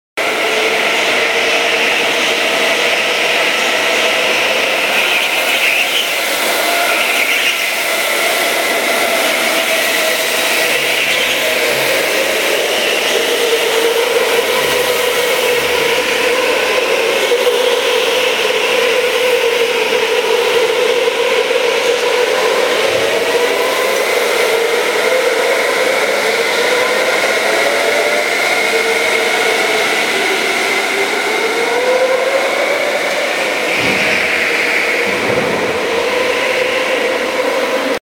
goparc circuito 01
Il circuito di Goparc a Mantova. Goparc è il kartodromo indoor più grande d'Europa.